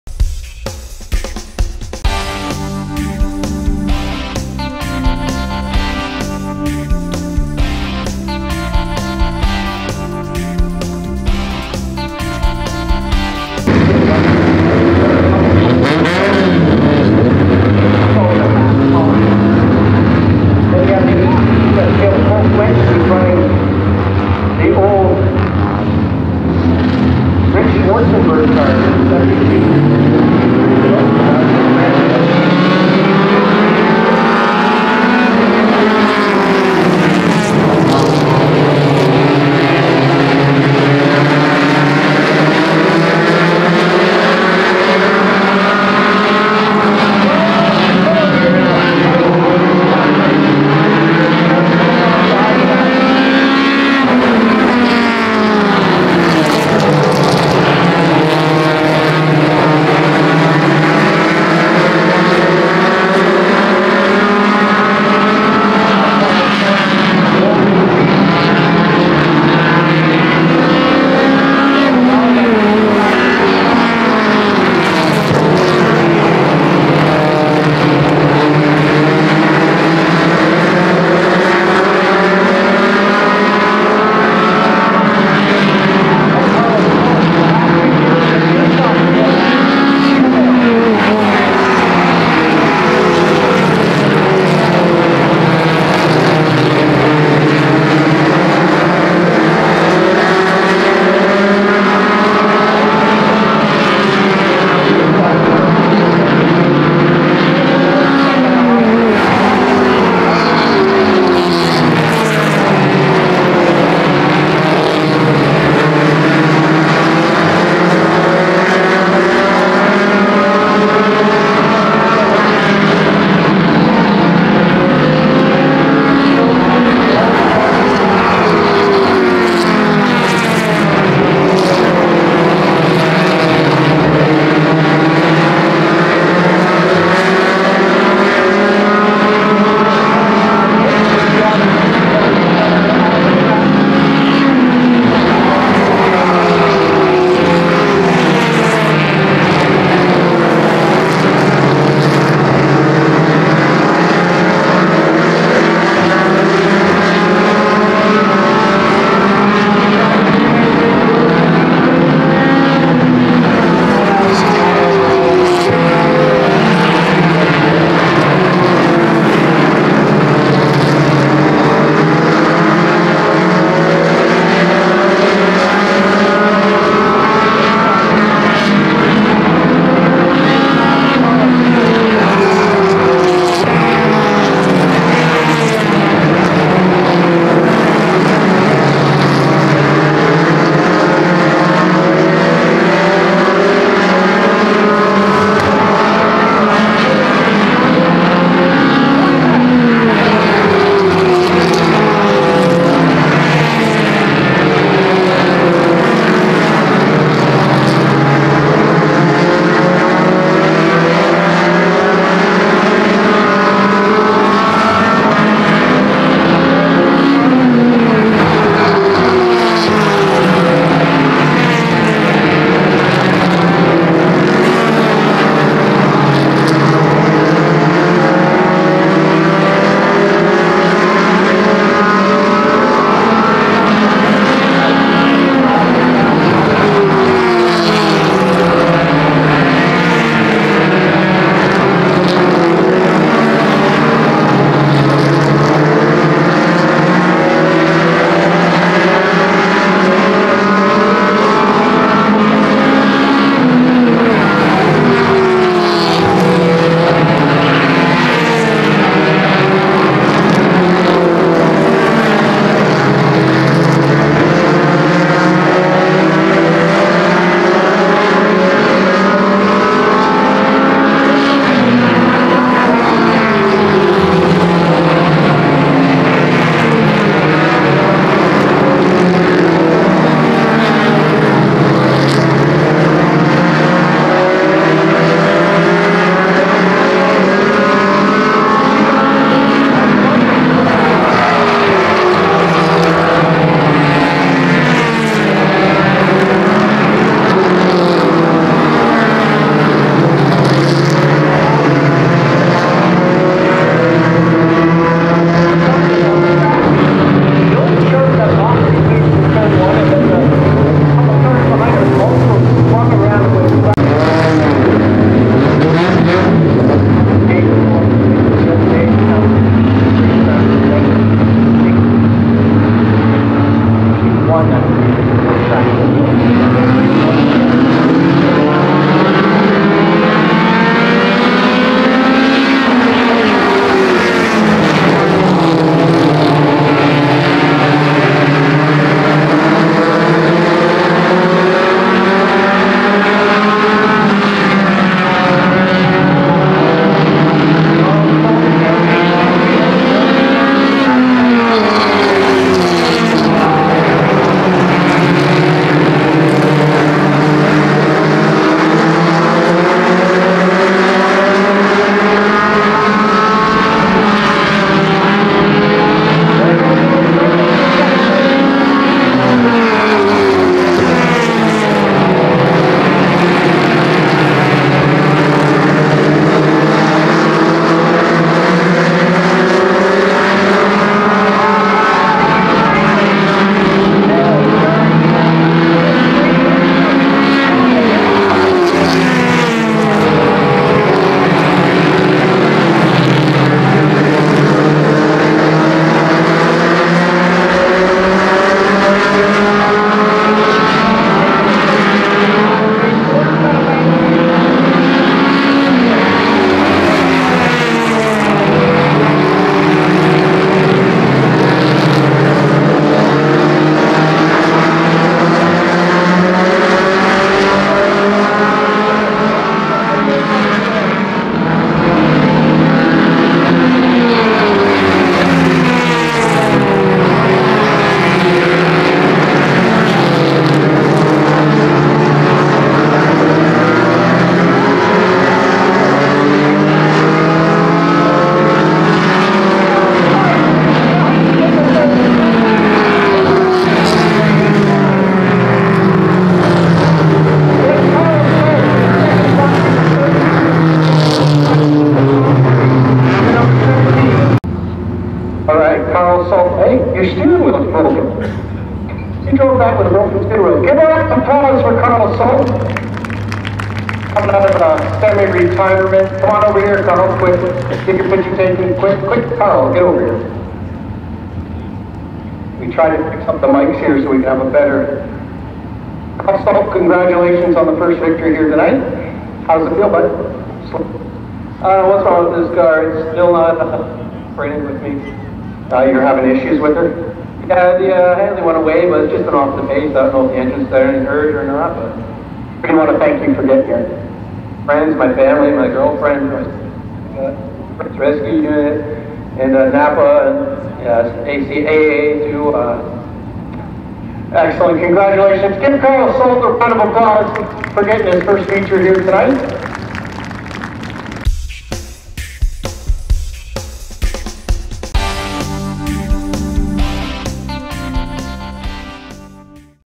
Flamboro Speedway, Mini Stocks, August 7, 2021
Flamboro Speedway, 1st feature for the Mini Stocks
winner  #81 Karl Sault
2nd      #32 Gillian Hills
3rd       #17 Dylan Sharpe